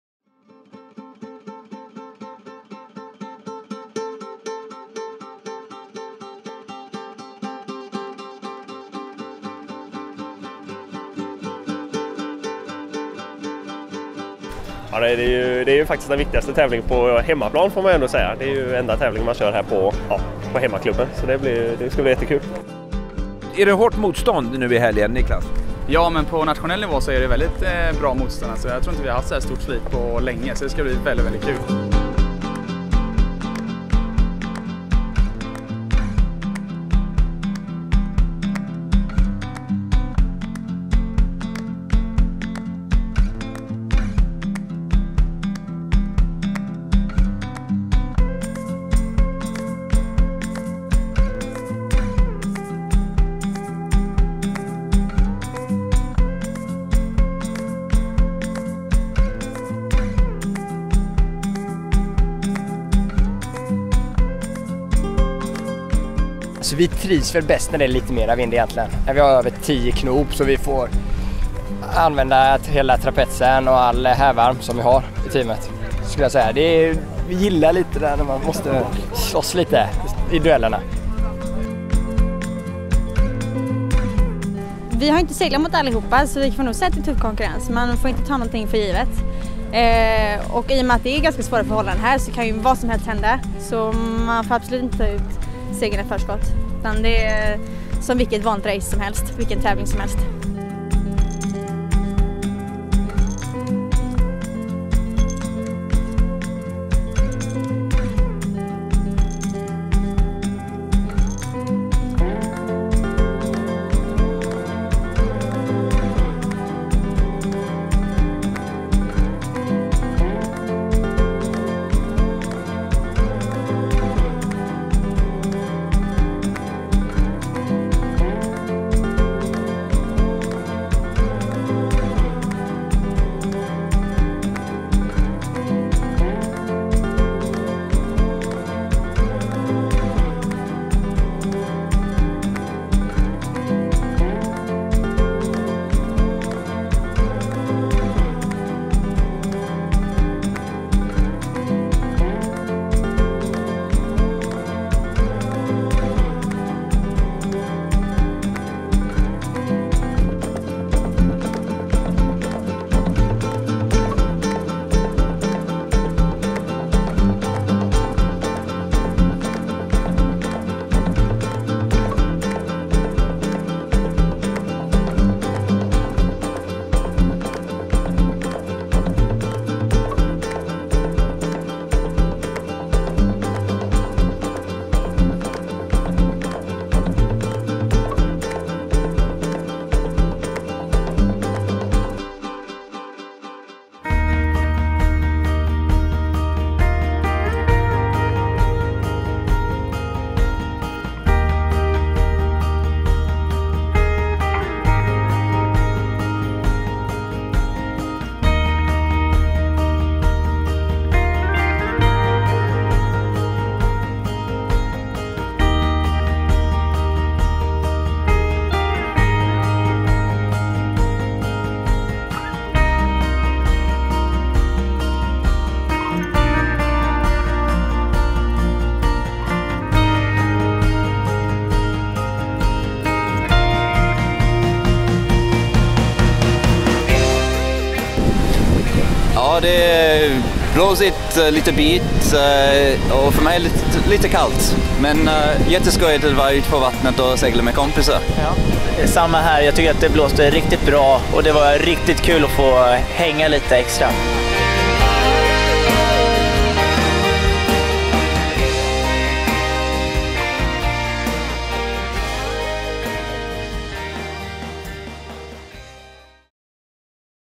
Race Day 1 (Saturday)
Årets final på KSSS seglingssäsong bjöd på tuff och tät segling under lördagen i vindbyar upp mot 10-12 sekundmeter. Förutsättningarna passade tydligen de finska seglarna bra som är i ledning i flera av klasserna.

KSSS Olympic Class Regatta är en viktig tävling, inte minst för KSSS egna elitseglare. I OS klasserna har Tim Shuwalow och Hanna Klinga, KSSS dominerat i Nacra klassen med fyra vinster och är i ledning. Joonas Lindgren, Helsingfors Segelklubb leder 470 klassen med tre spikar före Anton Dahlberg, KSSS och i Laser har Philip Claesson, MSS seglat till sig en ledning via tre jämna seglingar i hård kamp med Jesper Stålheim, KSSS som ligger tvåa inför söndagens avslutande seglingar.